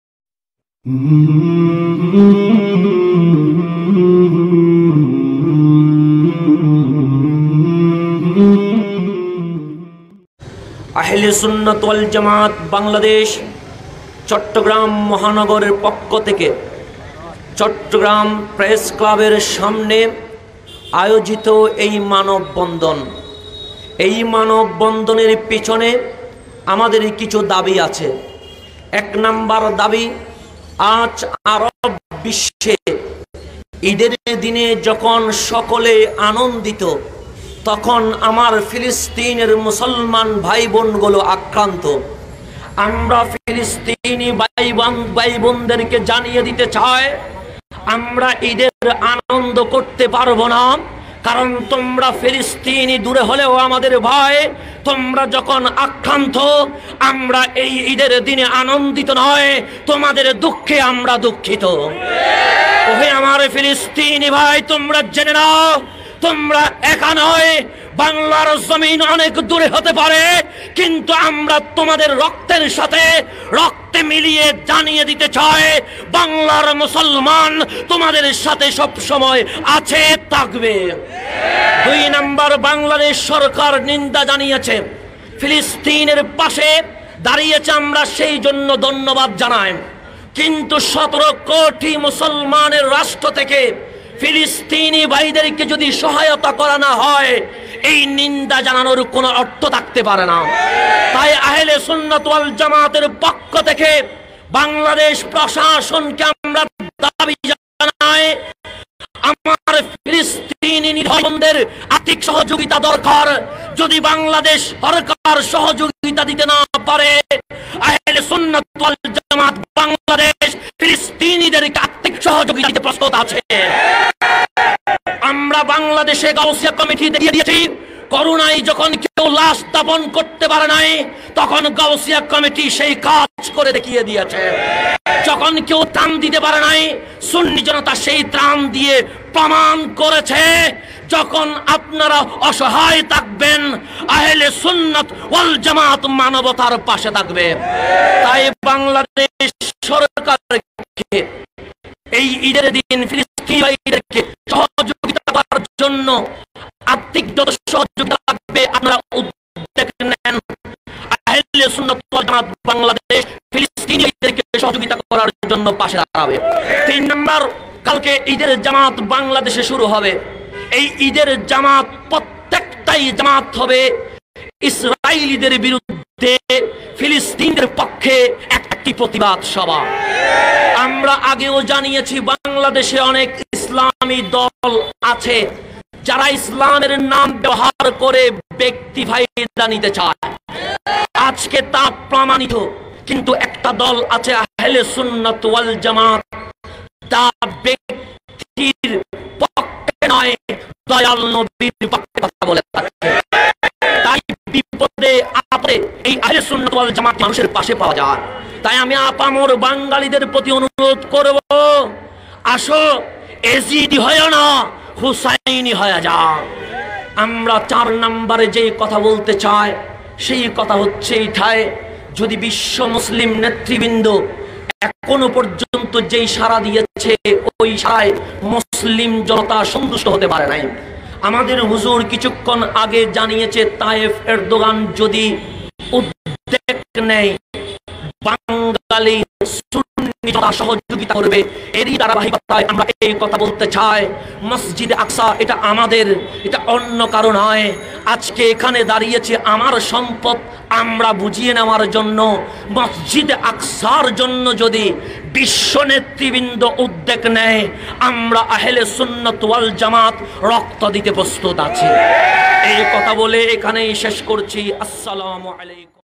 মসজিদে আকসায় হামলার প্রতিবাদে হাসান আজহারীর হুংকার || Save Palestine || Syed Hasan Azhari
শায়খ আল্লামা সৈয়দ হাসান আজহারীর হুংকার!!!

অনলাইনে ইসলামের সঠিক রূপরেখা প্রকাশ করতে আমাদের এই ক্ষুদ্র প্রয়াসে আপনার সহযোগিতা একান্ত কাম্য।